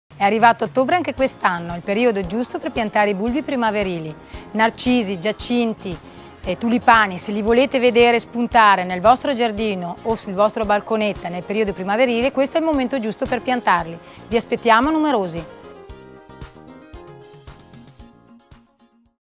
Bulbi a fioritura primaverile
Ottobre è il periodo perfetto per piantare i bulbi a fioritura primaverile!
La nostra Lara ti aspetta con tantissimi consigli per prepararti a una primavera piena di colore!